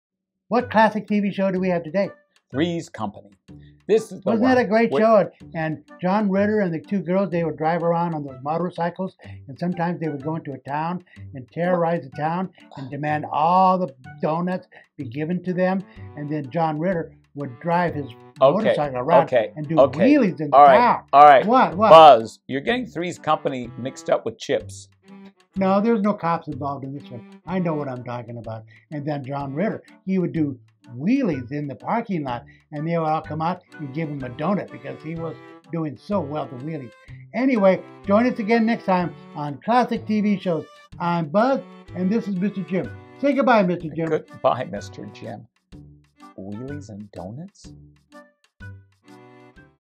Classic TV Threes company
Mr Jim and Buz review classic TV Threes company